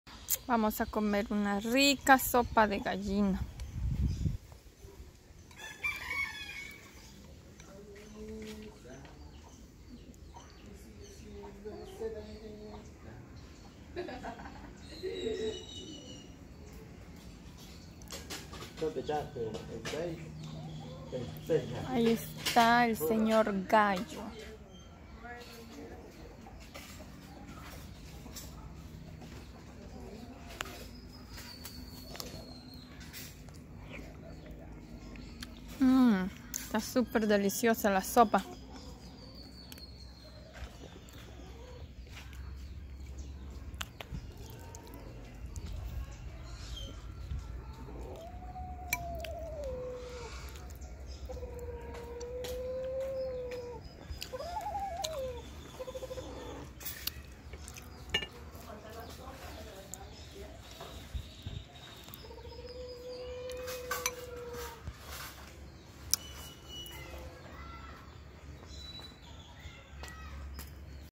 comiendo una rica sopa de gallina india en apancoyo sonsonate el salvador
compartiendo una de las más sabrosas sopas de gallina india salvadoreña